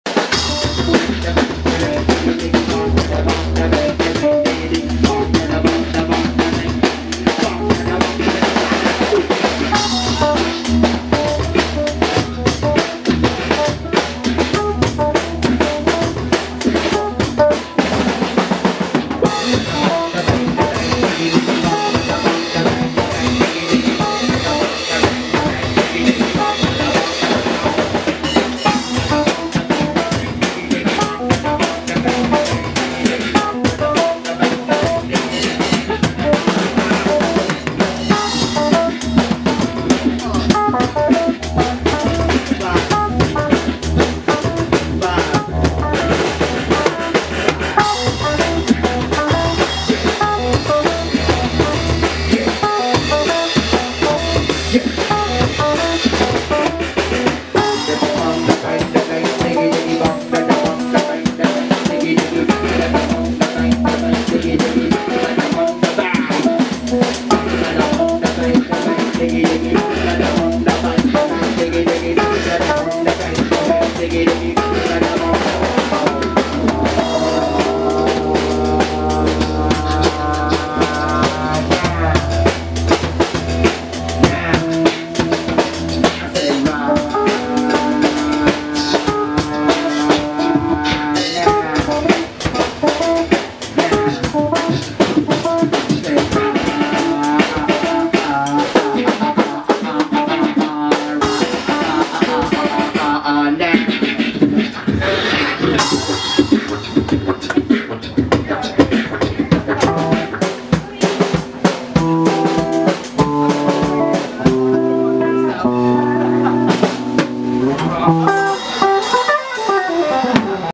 Africa Express Krar Collective & Kareem Rush & Reeps 1 & Romeo Stodart
Africa Express rehearsal - Krar Collective, Reeps One, Kareem Rush (Arabian Knights), Romeo Stodart (Magic Numbers) and others.

Recorded by Manuel Toledo, BBC Africa.

To follow the journey step by step, please where we have uploaded hundreds of pictures of the journey